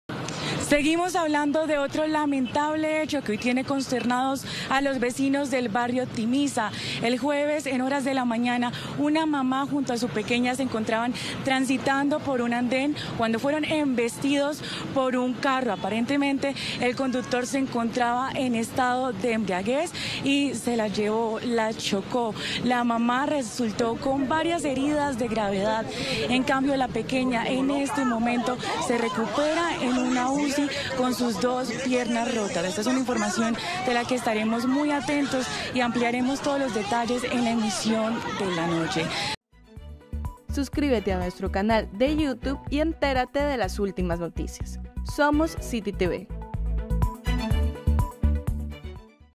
Conductor en presunto estado de embriaguez le fracturó las piernas a una menor de 8 años | CityTv
En la mañana del pasado jueves 17 de agosto se registró un grave accidente en el barrio Timiza. Una camioneta embistió a la niña de ocho años y a su mamá, dejando a la menor con las piernas fracturadas y en delicado estado de salud. Actualmente se encuentra en cuidados intensivos. Presuntamente el conductor del vehículo se encontraba en estado de alicoramiento. Todos los detalles a continuación.